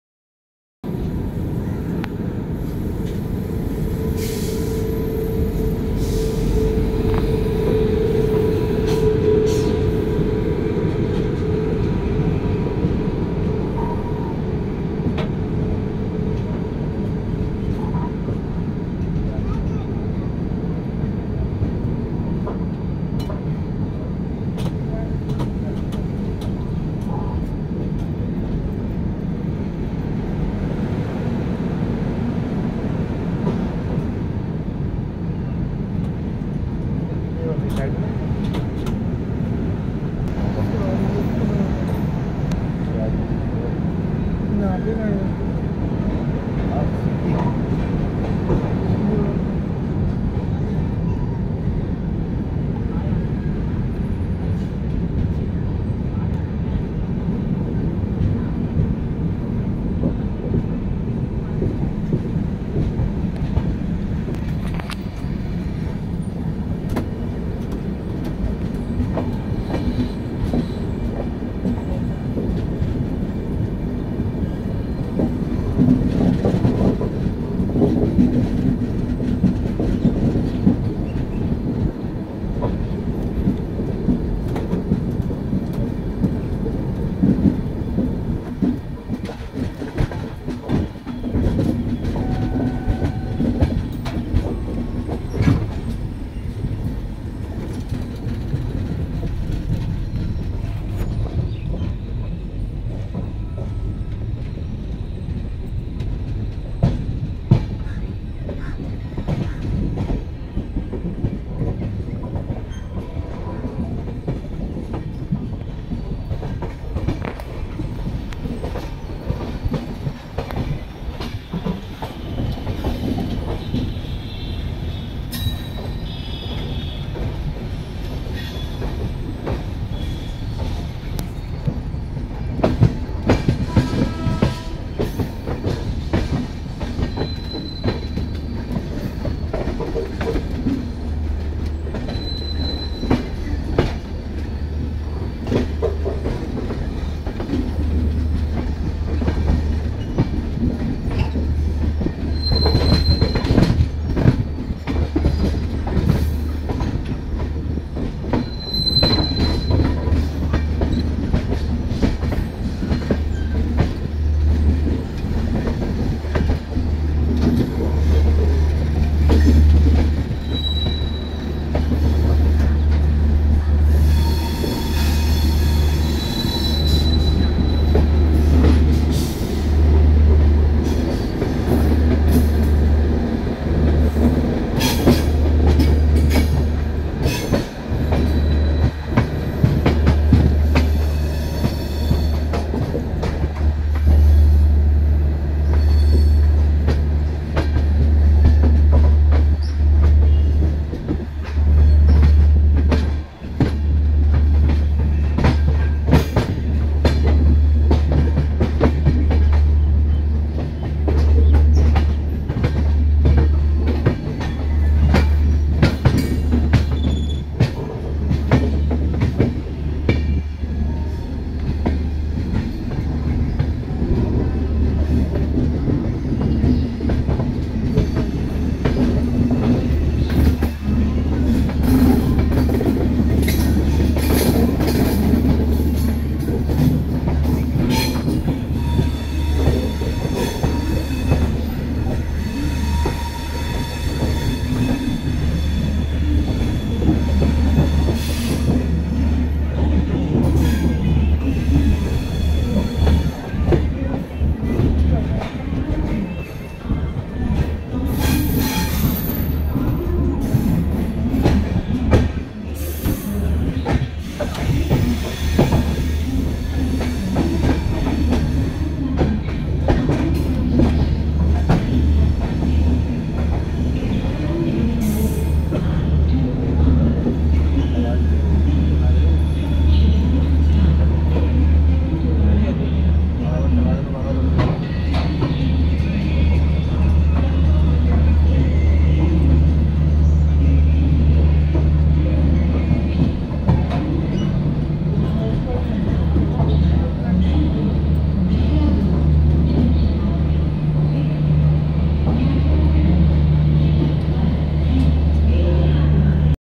शिवाजी ब्रिज से निकल कर गोल घुमते हुए नई दिल्ली पहुंची सचखंड एक्सप्रेस।
Onboard 12715 Sachkhand Express.
Arriving New Delhi Station
Meets  New Delhi Jalandhar City Intercity Express Meets At Shivaji Bridge.
Nd Shunting Ranchi Garibrath Express At New Delhi.